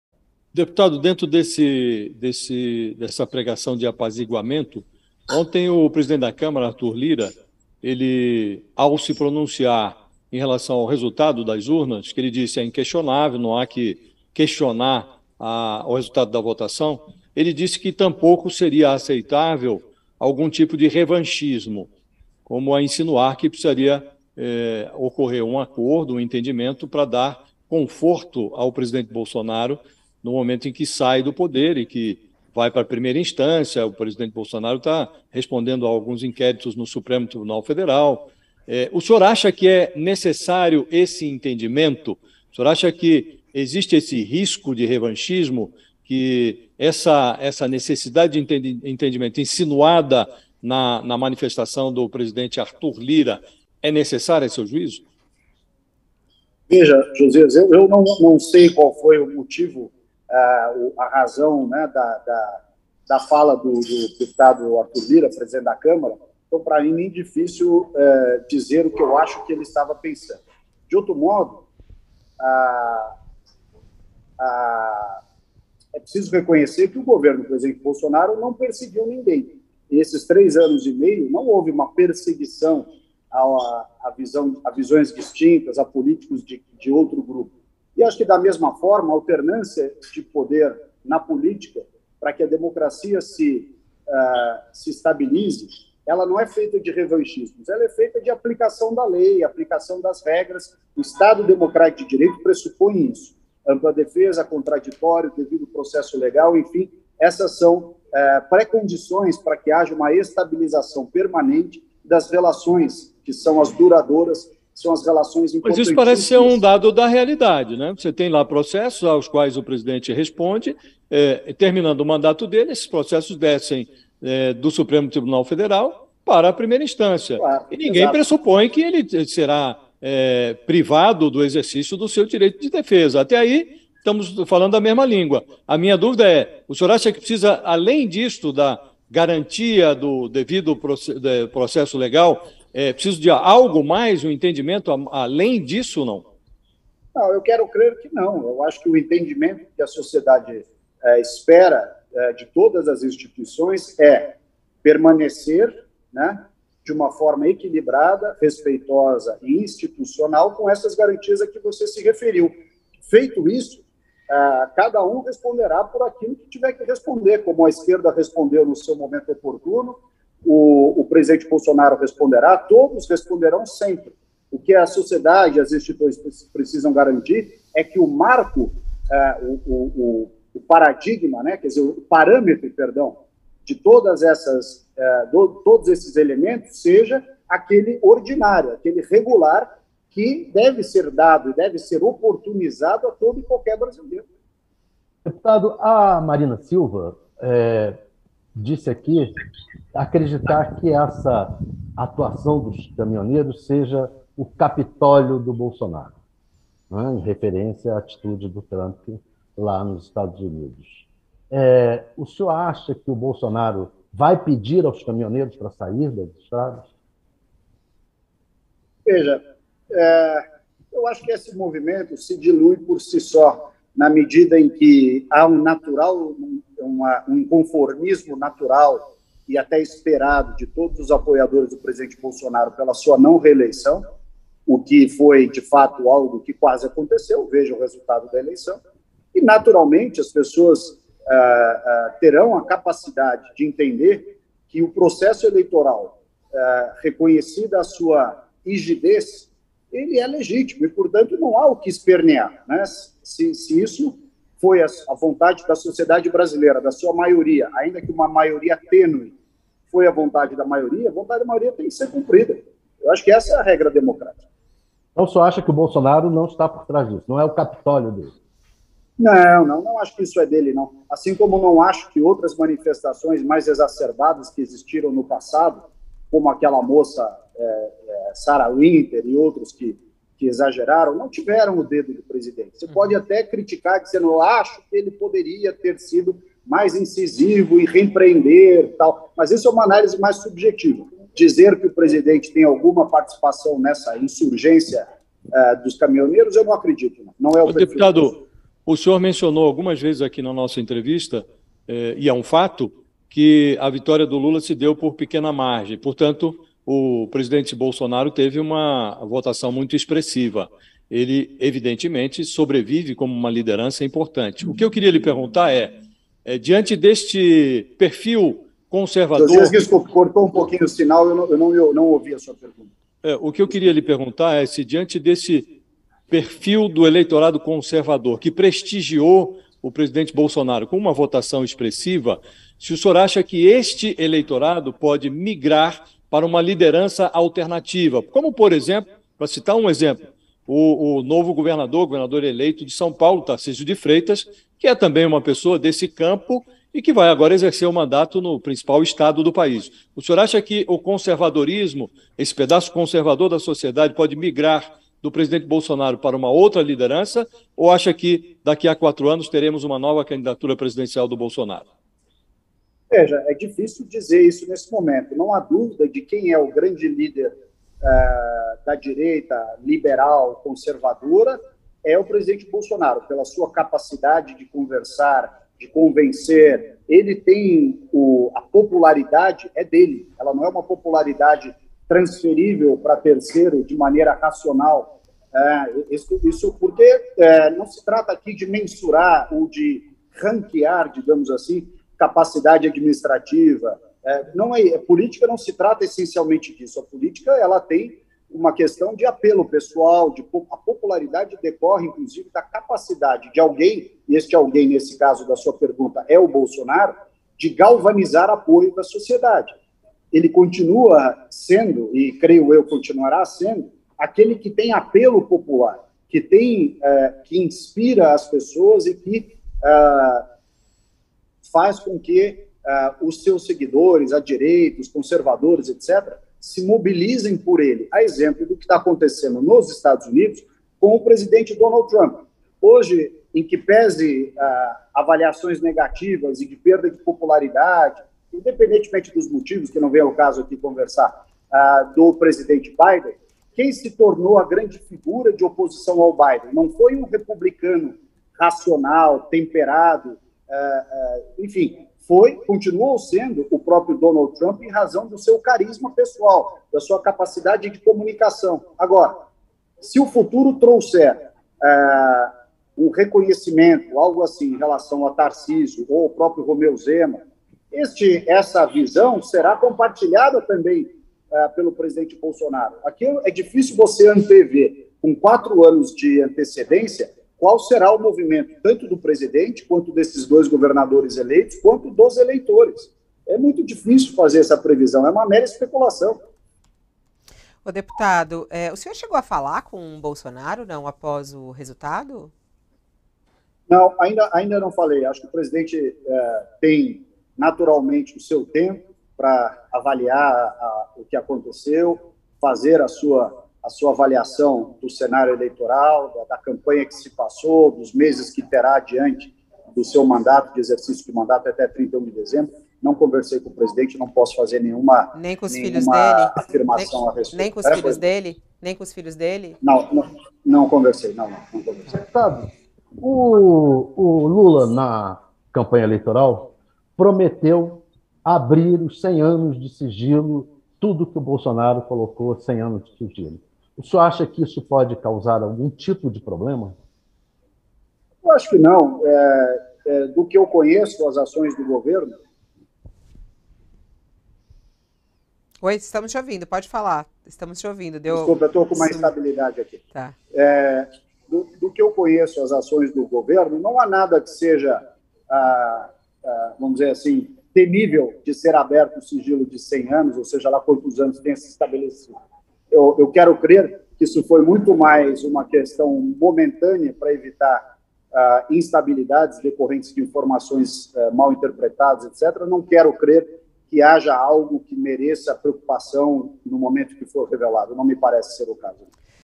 Salles diz que 'Bolsonaro responderá assim como a esquerda': 'Todos responderão sempre'
Em entrevista ao UOL News, o ex-ministro de Bolsonaro e deputado federal eleito Ricardo Salles fala sobre a eleição de Lula, os protestos de caminhoneiros e o governo em 2023